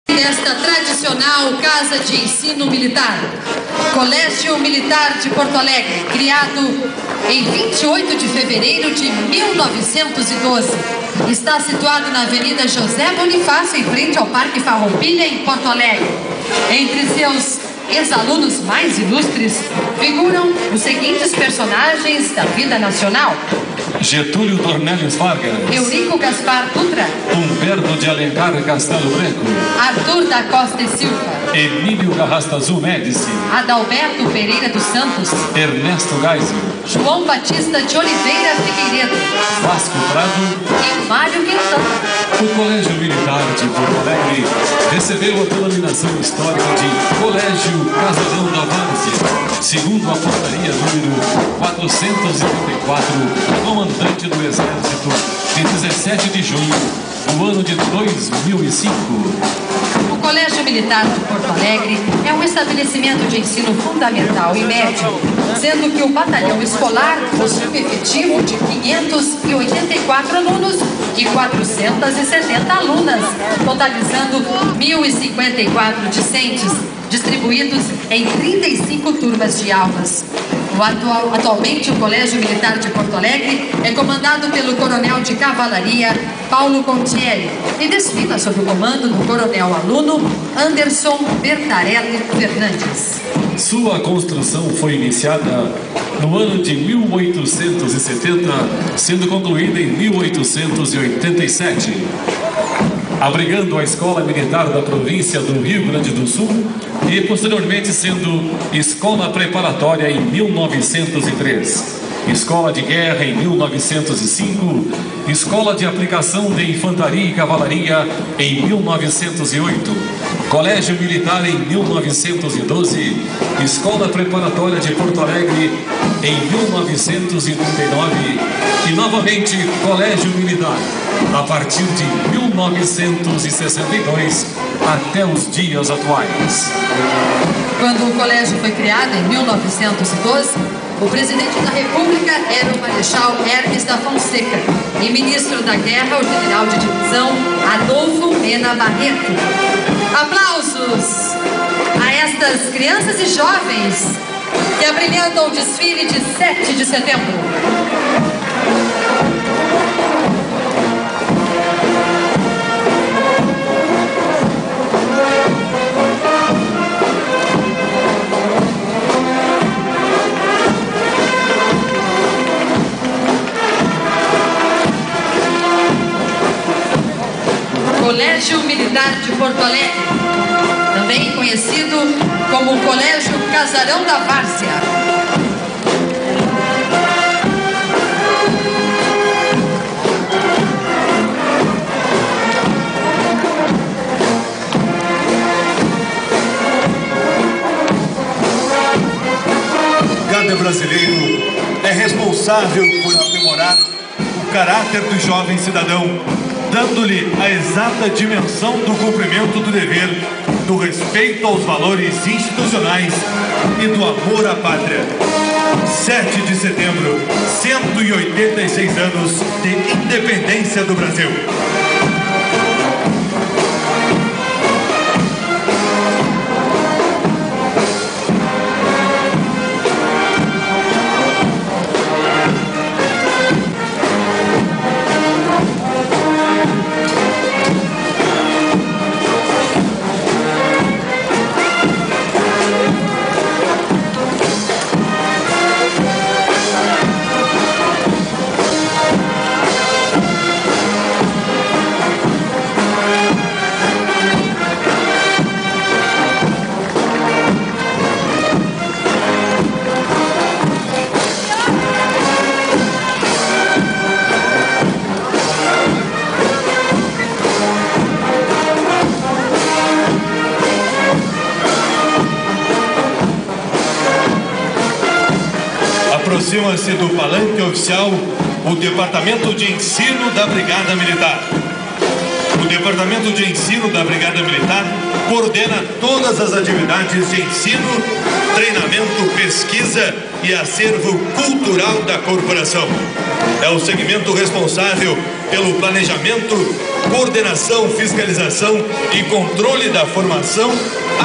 CMPA - Desfile de 7 de Setembro 2008
CMPA - Desfile de 7 de Setembro 2008, gravado pelo Maj Giovani Correa Mascarenhas da Silva.